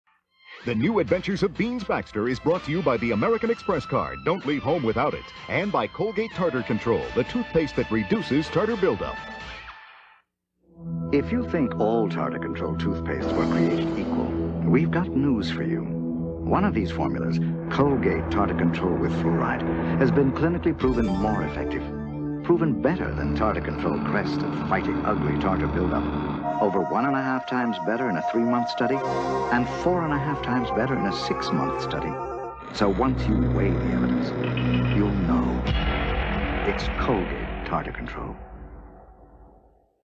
Colgate Tartar Control Formula Commercial (1987)
Compare this toothpaste over Tartar Control Crest.